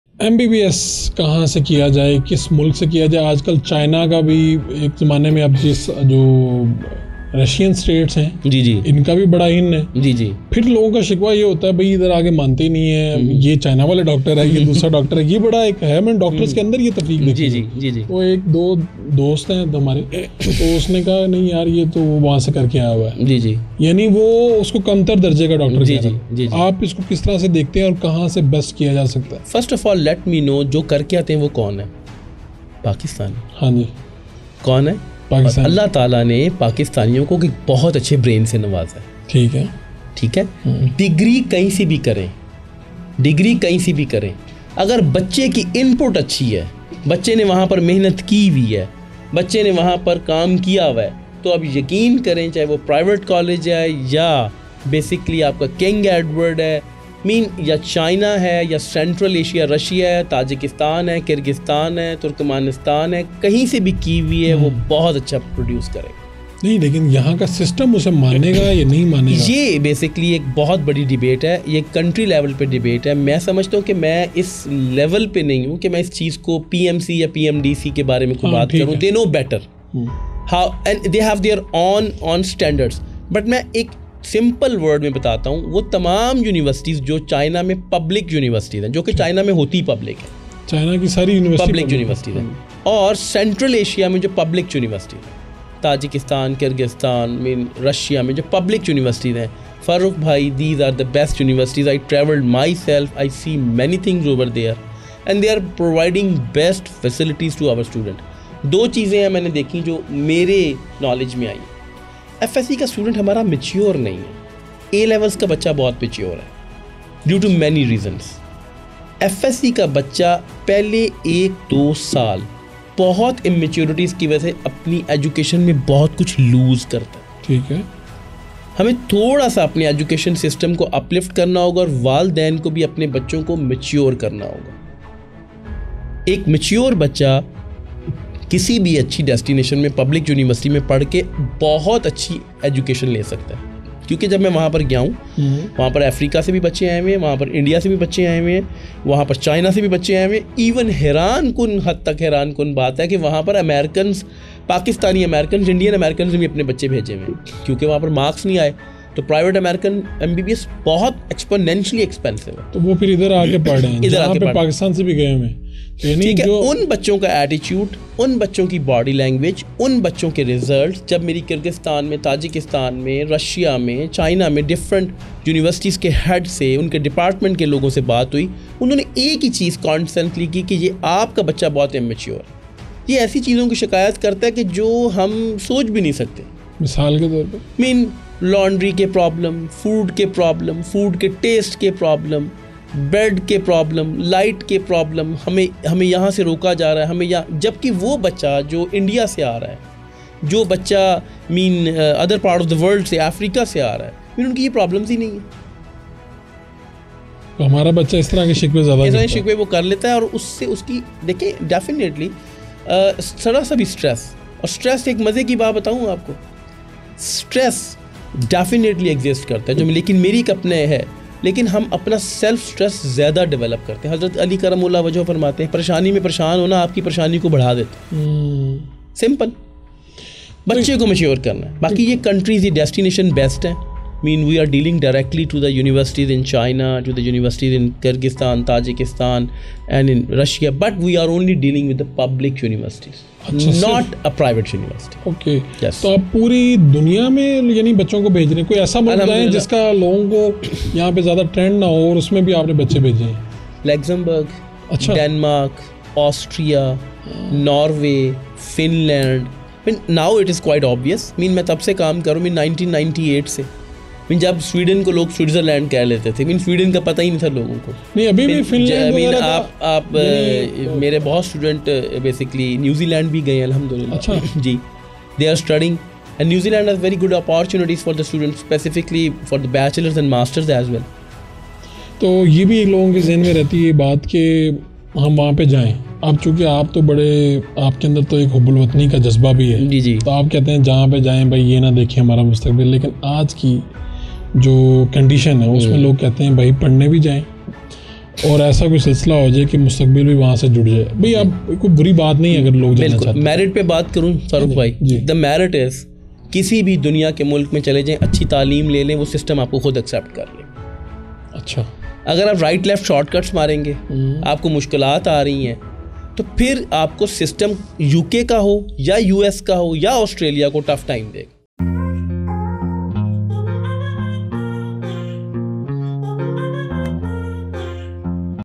MBBS ki Studies ke liye China kaisa Mulk ha? | Dr. Taimoor Ali Ahmed | Farrukh Warraich
► Host: Farrukh Warraich
► Guest: Dr. Taimoor Ali Ahmed